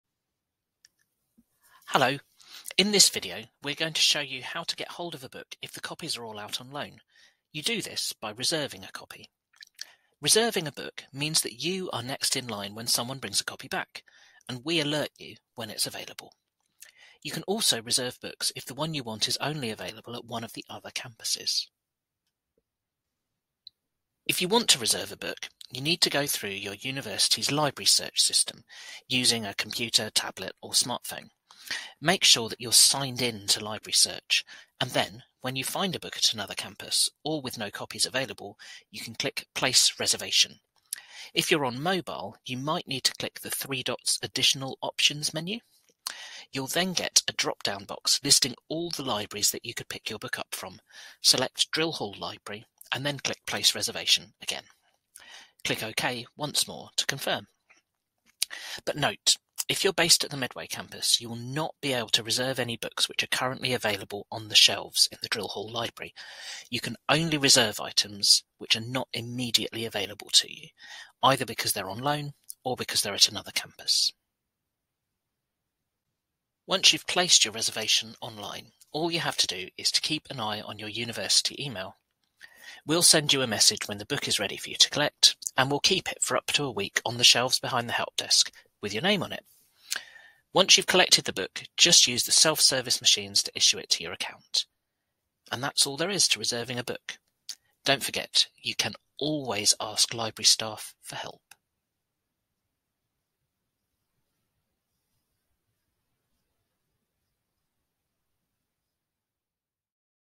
Reserving books
A short overview of how to reserve books for collection at the Drill Hall Library.